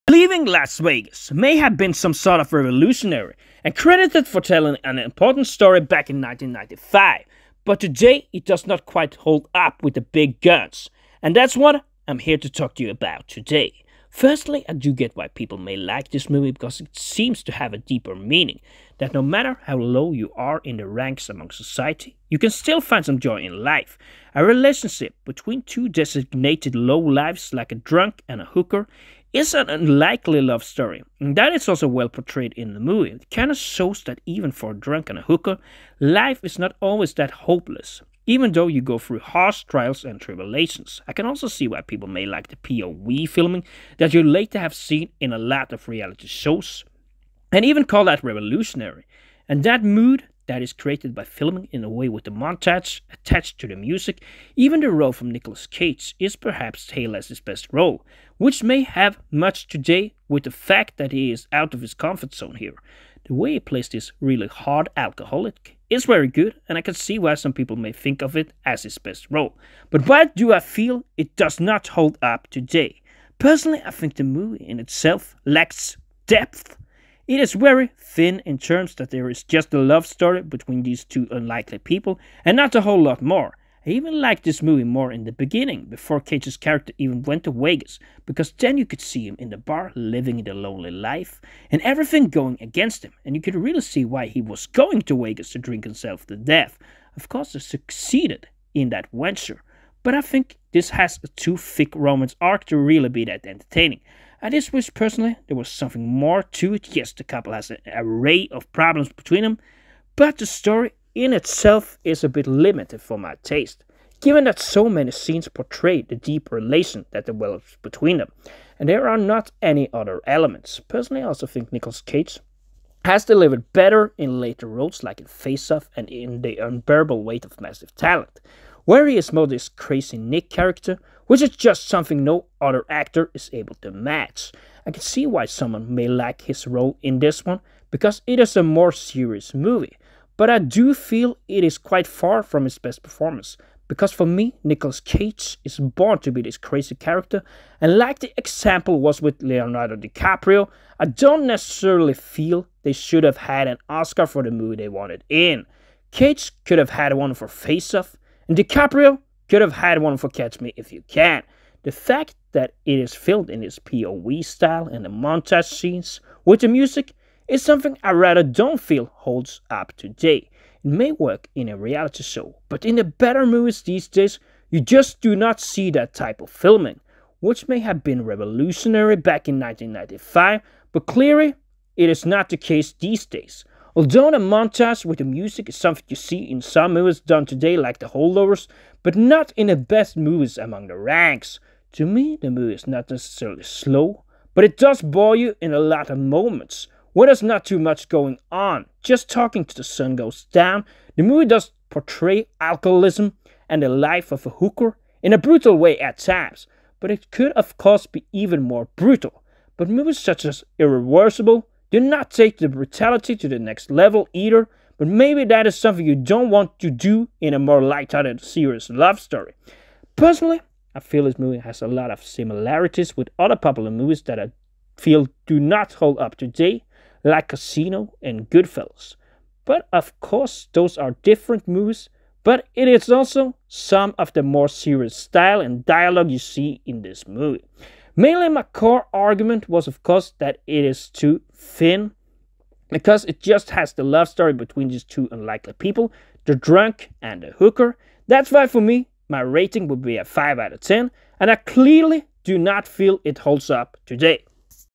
Why Leaving Las Vegas Does Not Hold Up Today
Why Leaving Las Vegas Does Not Hold Up Today                                                                                                        Today I will talk to you about on why Leaving Las Vegas does not hold up today. One of the reasons why Leaving Las Vegas does not hold up today is because the story is quite thin, there's a love-story but not any other elements. In why leaving Las Vegas does not hold up today I personally feel it has a lot of similiarties with other popular movies that I feel do not hold up today like Casino and Goodfellas.